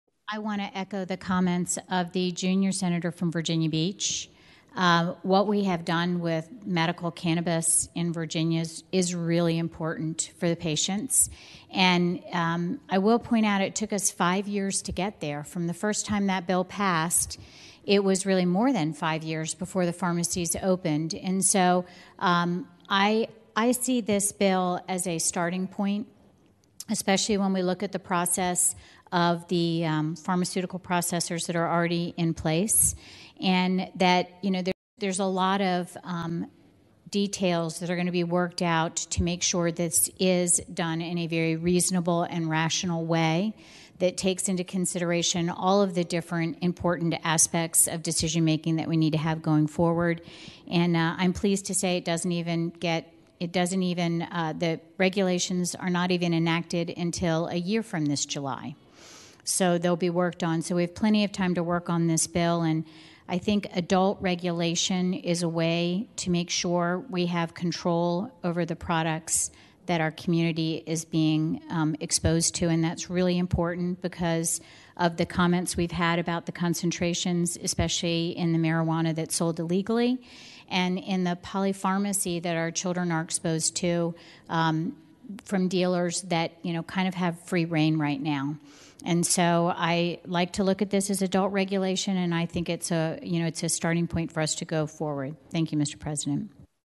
Senator Siobhan Dunnavant Speaking to SB1406
Senator Siobhan Dunnavant speaking to SB1406 on the Senate floor.